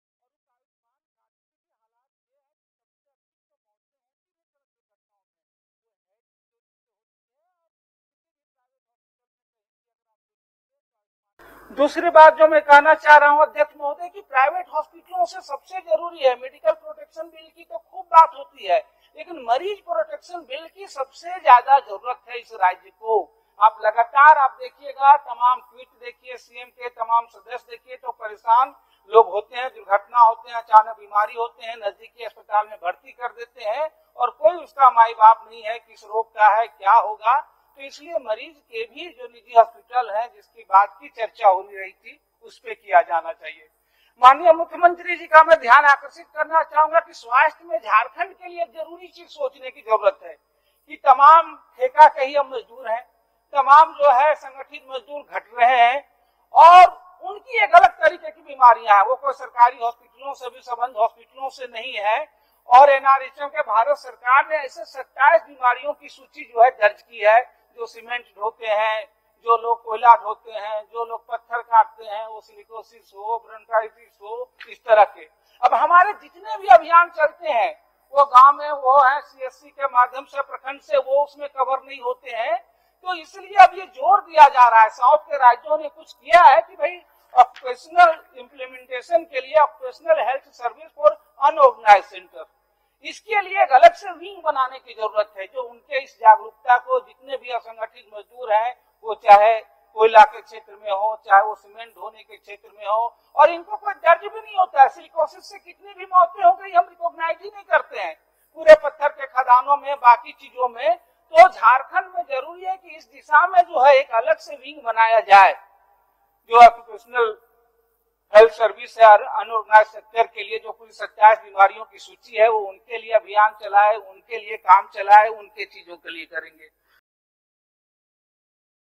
Silicosis and health issues are raised by the MLA Vinod Kr Singh in the State Assembly on 17/03/2021
Silicosis and health issues are raised by the MLA Vinod Kumar Singh in the Jharkhand State Assembly on 17th March 2021.